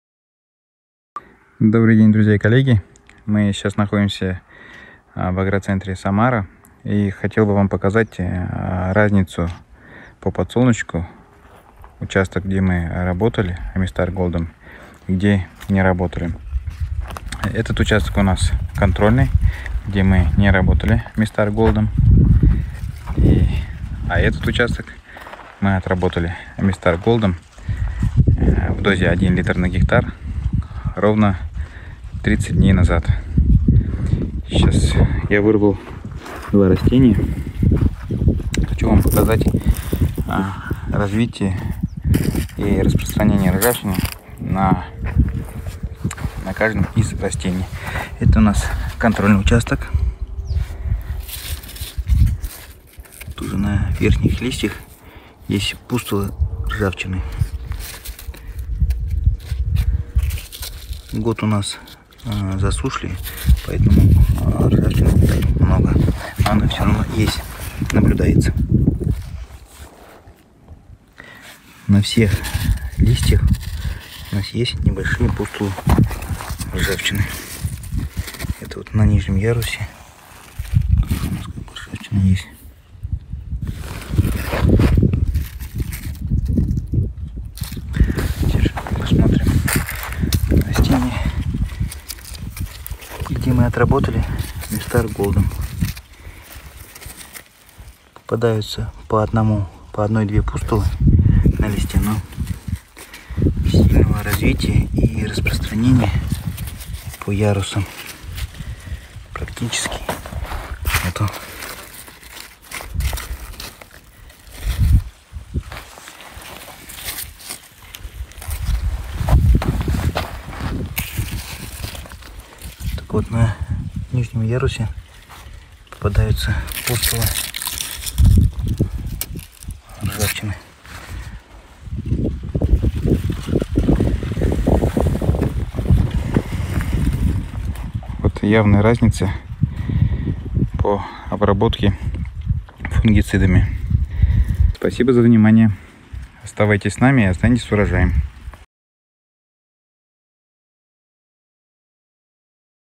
Фунгицид на подсолнечнике, Самара
Применение фунгицида на подсолнечнике в засушливый год - оцениваем эффективность защиты